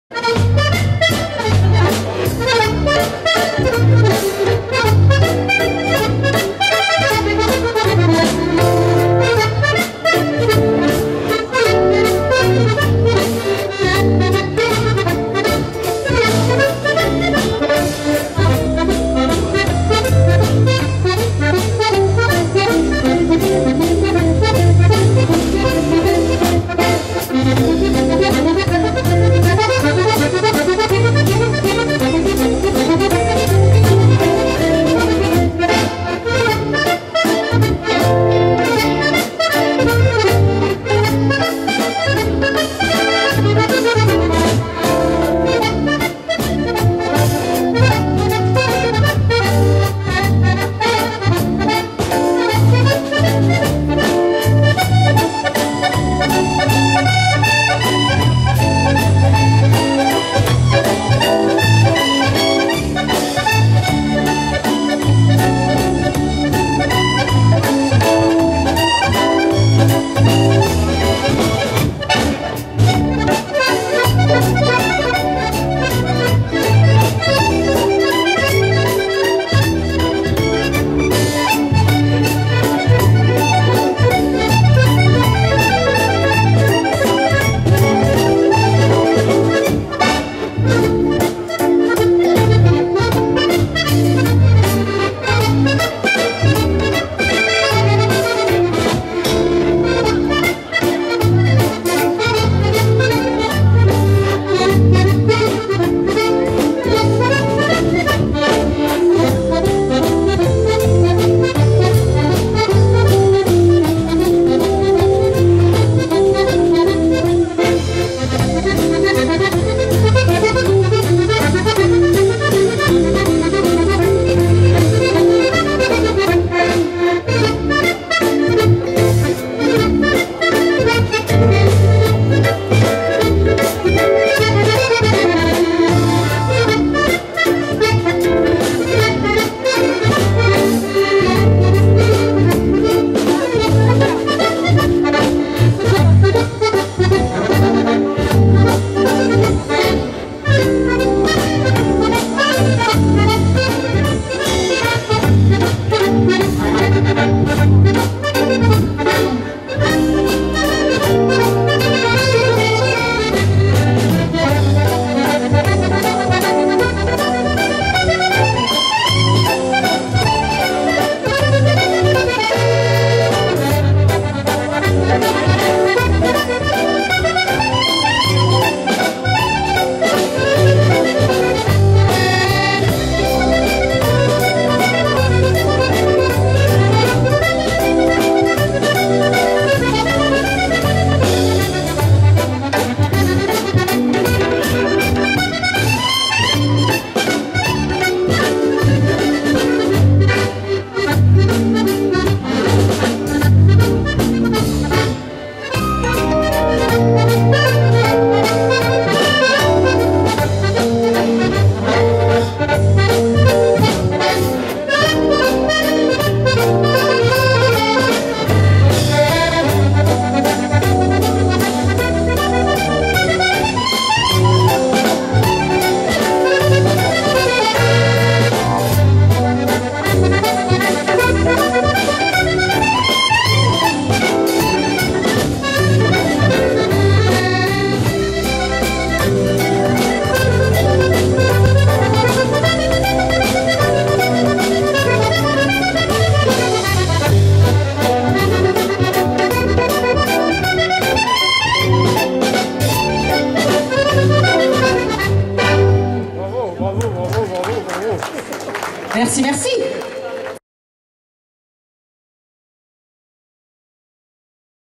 SANDRINE TARAYRE AURILLAC(15)avril 2014 Java
Gala MUSIQUES et RYTHMES D'AUVERGNE AURILLAC(15) avril 2014/SANDRINE TARAYRE/Orchestre SYLVIE MAUBERT/Vidéo et réalisation Alexandra ASTORG "Sandy"